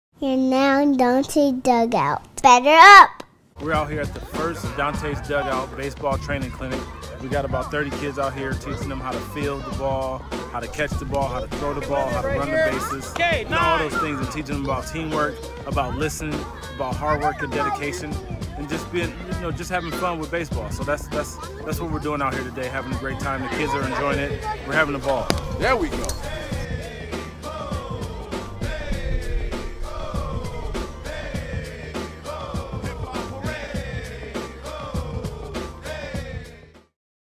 Dontay's Dugout Training Clinic
This is a short clip of the first Dontay's Dugout baseball training clinic.